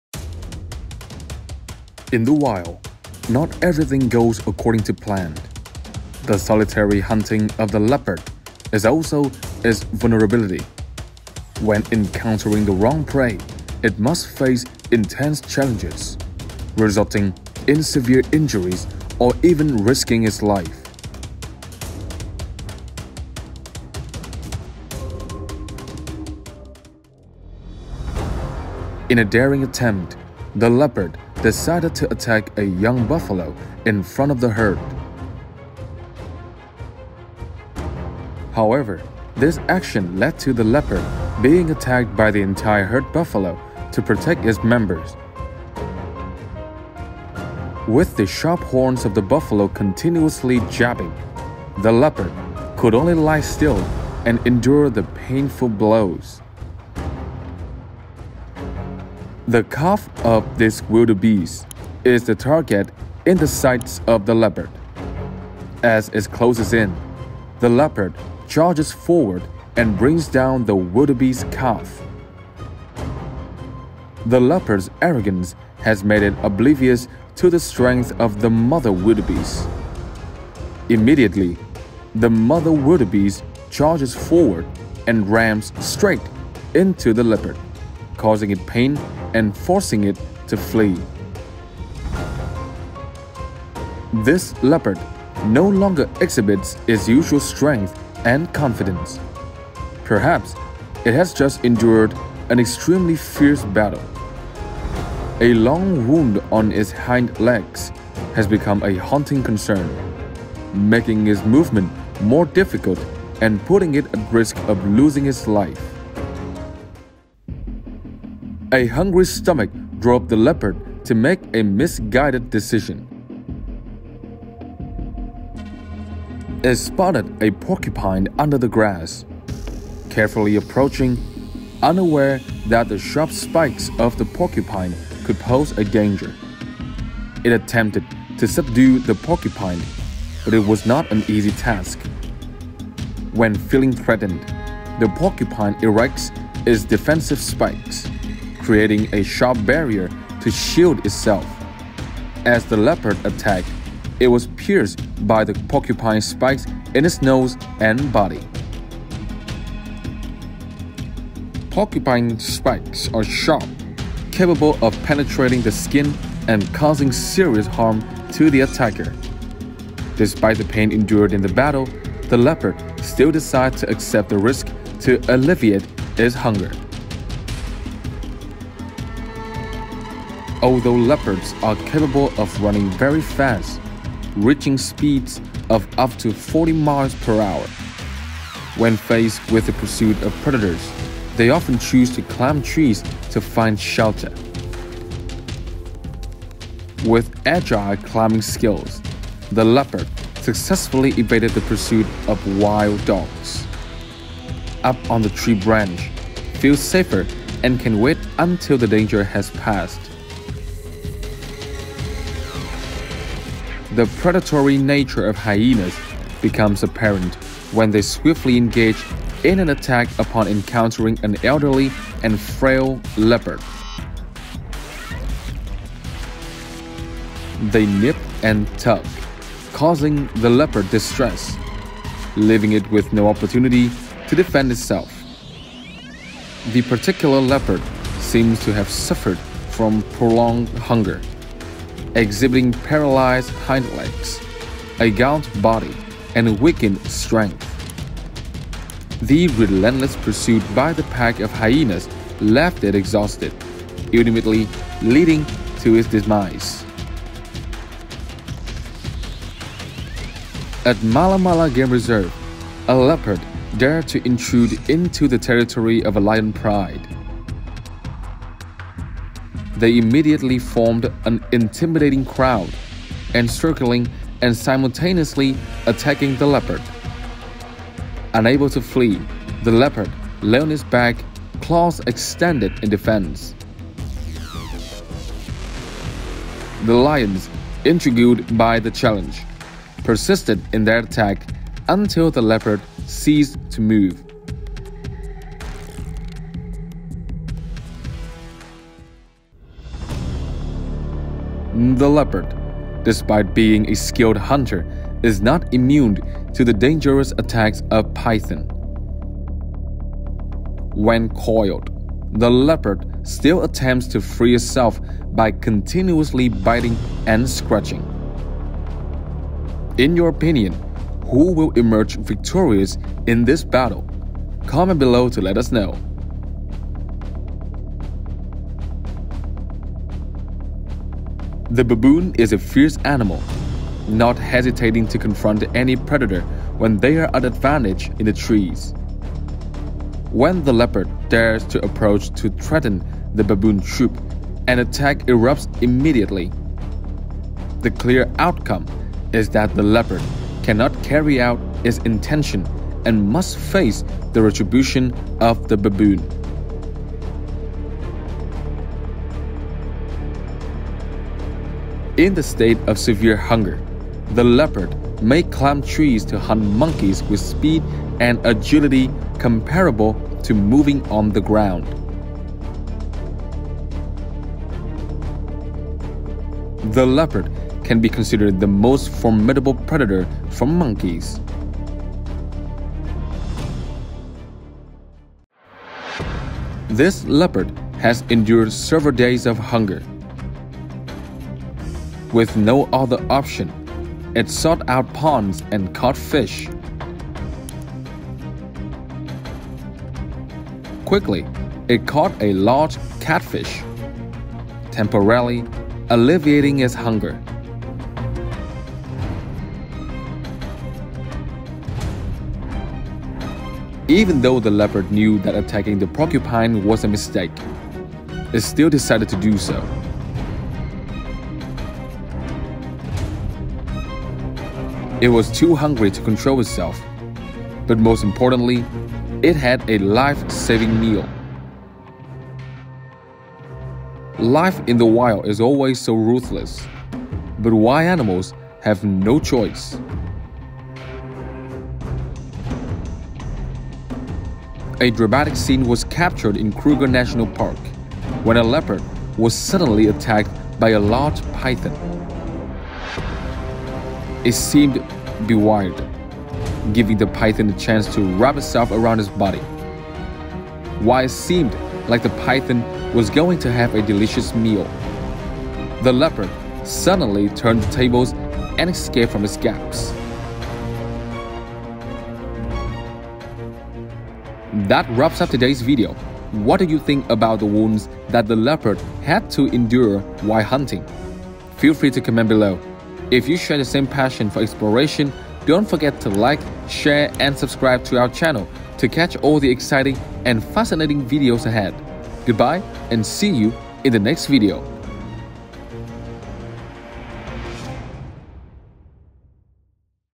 15 Bad Moments Leopards Get Injured While Picking The Wrong Prey | Wild Animals
15 Bad Moments Leopards Get Injured While Picking The Wrong Prey | Wild Animals | The Sense
In the natural habitat, the rhythm of life for wild animals is often unpredictable. The leopard's solitary hunting technique, while efficient, can also be a vulnerability. When faced with unexpected prey, it encounters formidable challenges, risking injury or worse.

In a bold move, the leopard targeted a young buffalo amidst the herd. Yet, this decision resulted in the entire herd rallying to protect its own. Enduring the relentless jabs of buffalo horns, the wild animal found itself in a precarious situation.

Fixated on a wildebeest calf, the leopard pounced, unaware of the mother's protective instincts. The ensuing clash left the leopard battered and fleeing, its confidence shaken.

Now, weakened and nursing a severe hind leg wound, the once formidable predator navigates its environment with newfound caution, its survival uncertain.